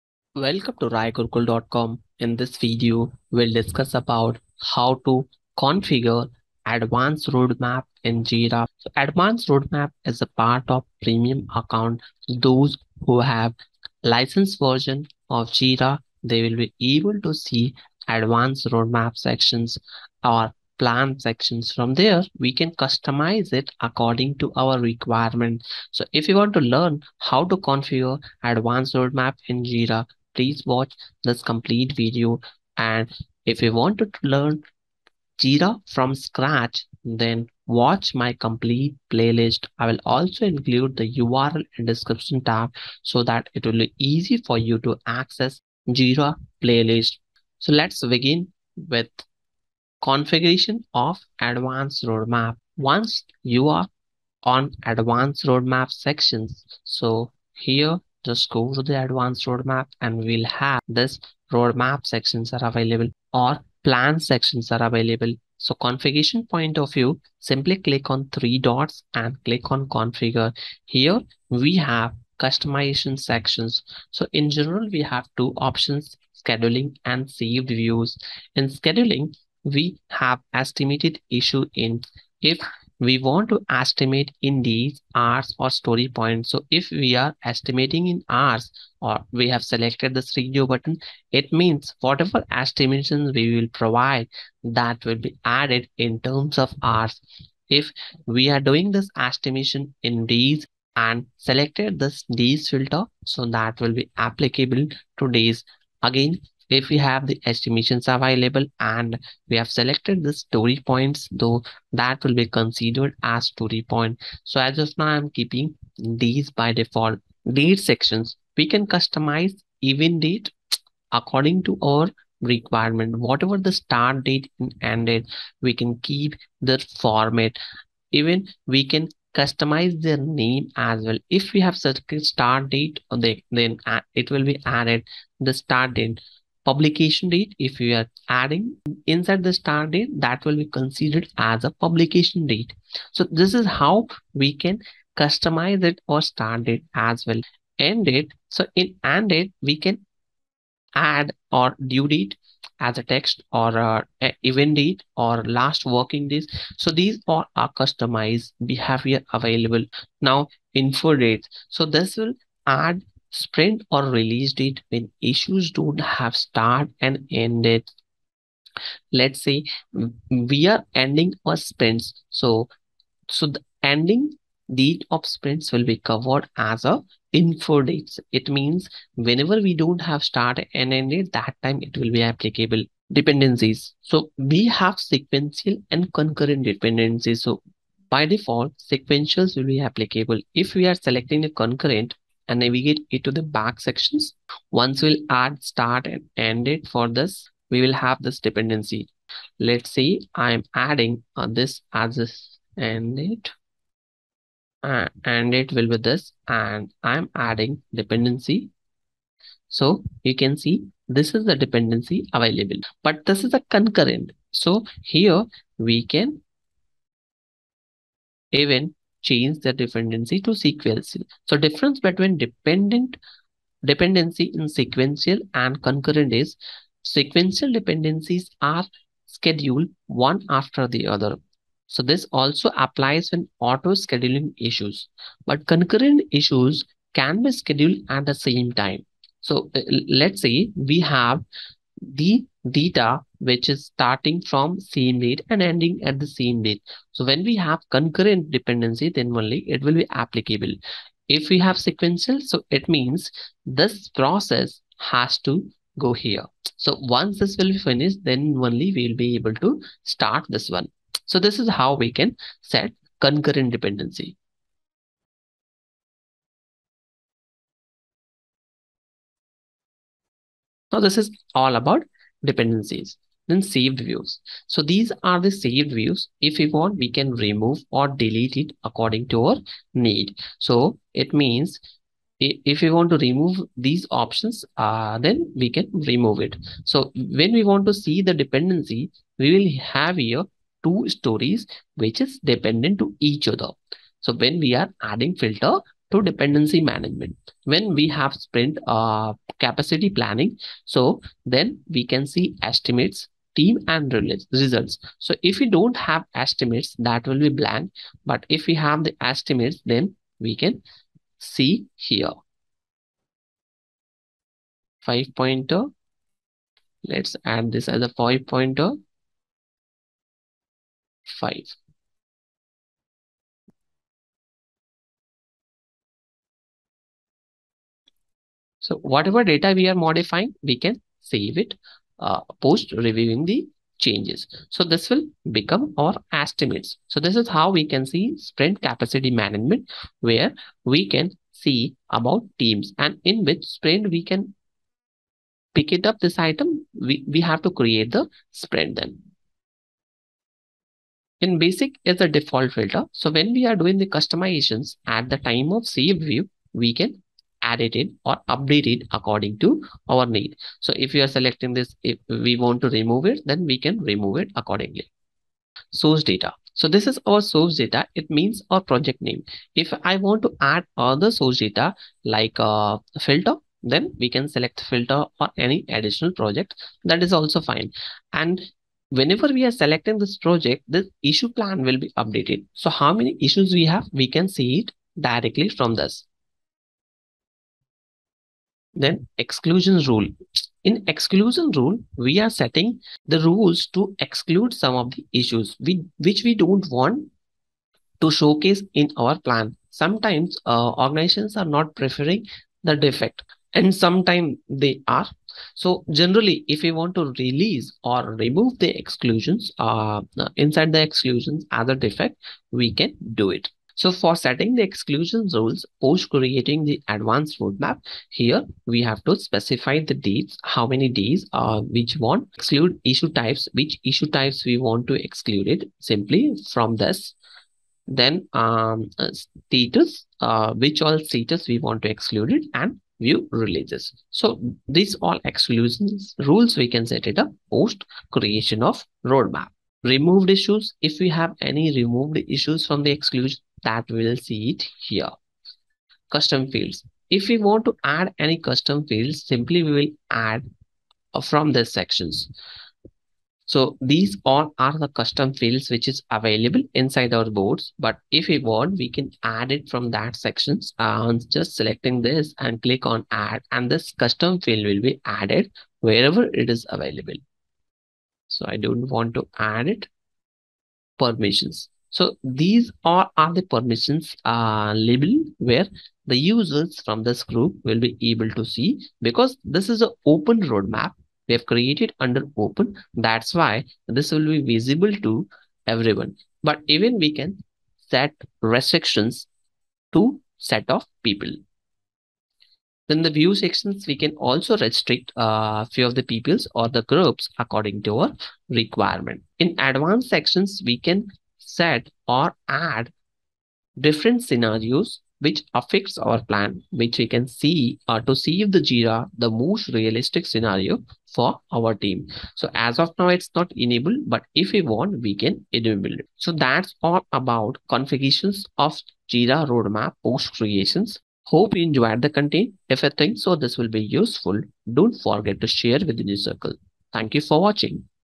How to Customize Advance Roadmap in Jira
Advanced Roadmaps for Jira Software allows you to plan and track work strategically across multiple teams and projects. Designed to empower teams at scale, you can plan based on capacity, track dependencies, manage competing priorities, and explore alternative scenarios with a single source of truth into the current and future health of your initiatives.